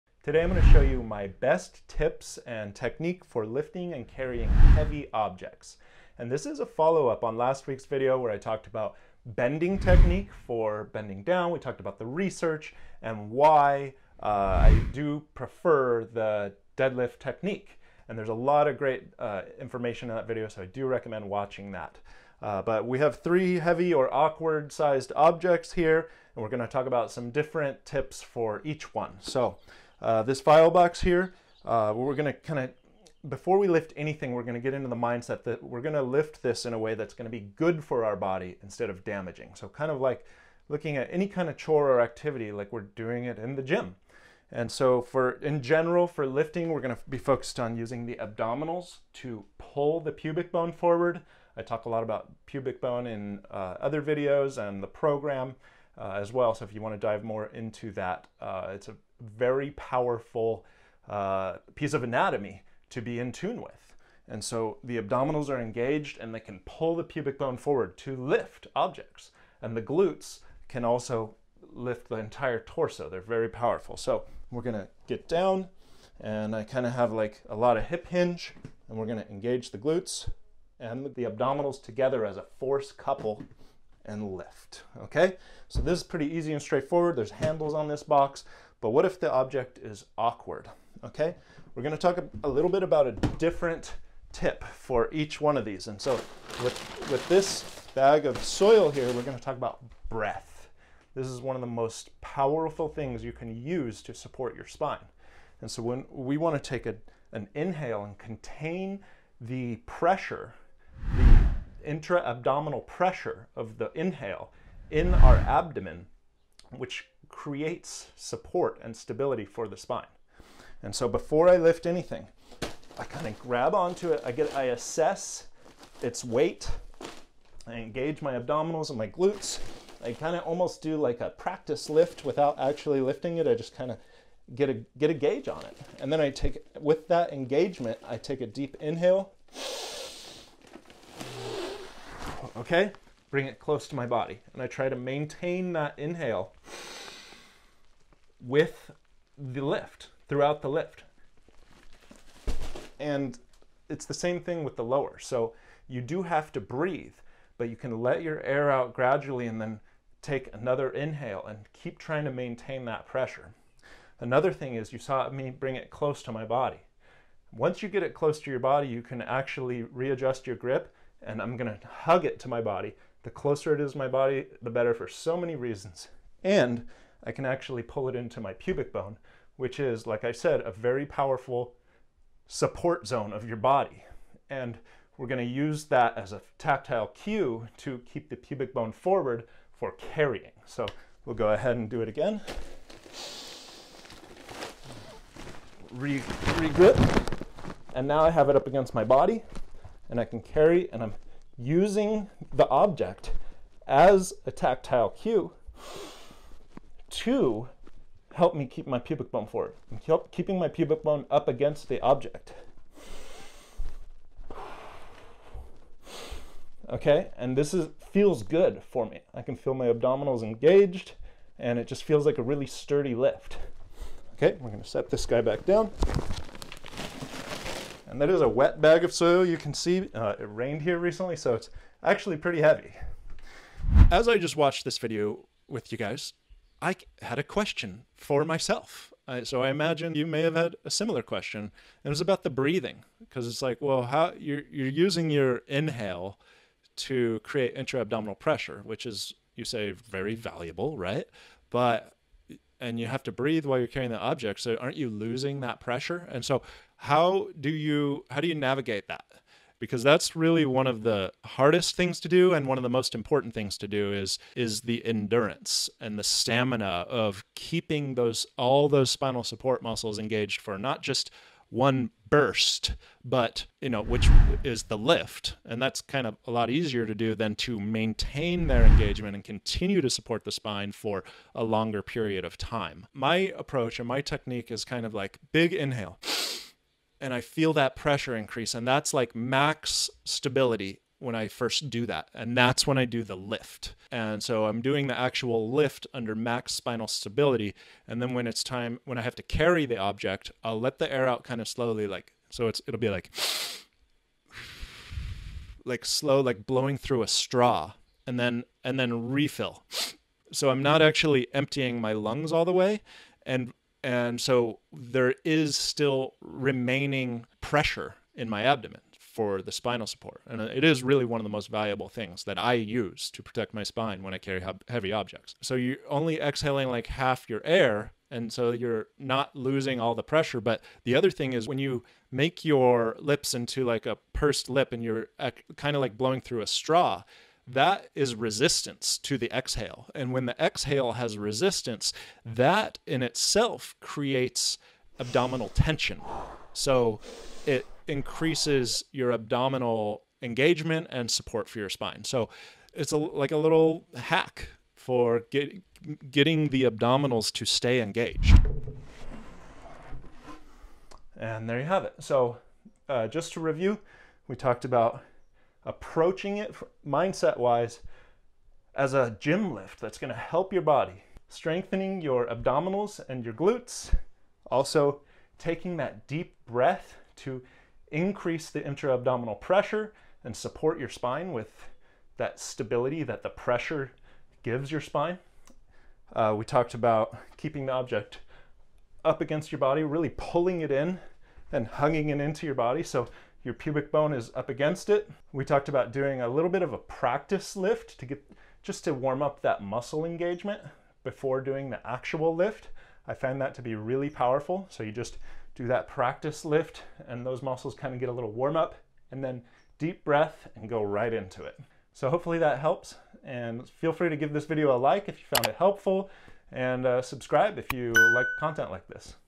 Best way to lift heavy objects SAFELY 🔴 without BACK PAIN
If you don't lift heavy objects safely, it can cause a lot of damage to your body, and cause severe back pain. We all have situations when we need to lift and carry heavy things, be it a piece of furniture or gardening equipment and supplies. It's important to know how to lift and carry them safely, and stay healthy and pain-free. In this video I explain how to do it and protect your spine.

BEST Exercise for Pain Between Shoulder Blades Relief 🔥